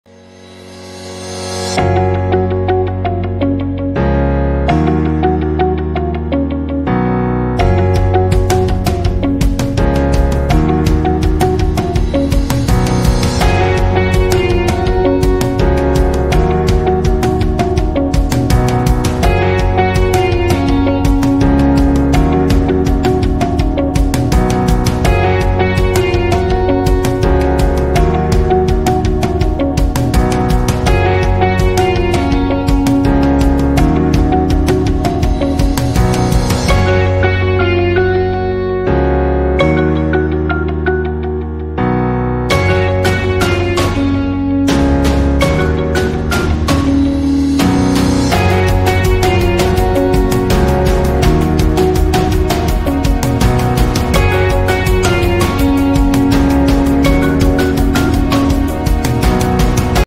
02/27/2023: Nightlapse: Lake Travis, Texas (Austin, TX): 4K, 60fps Daily Timelapse Video
4K, 60fps Daily Nightlapse Video taken near Lakeway, Texas (Austin, Texas)